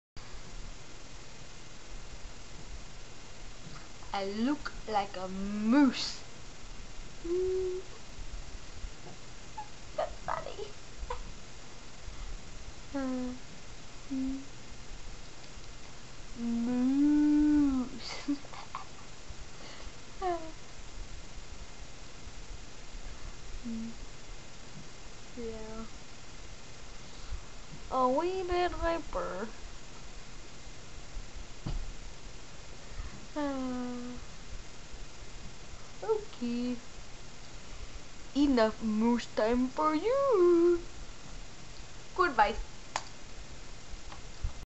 Moose time.
When I hit the record button, i didn't have any intentions on doing anything.
But, once I saw myself, yea, i looked like a MOOSE! :D
So, yea.

OH! I had a SUPER big gulp beforehand.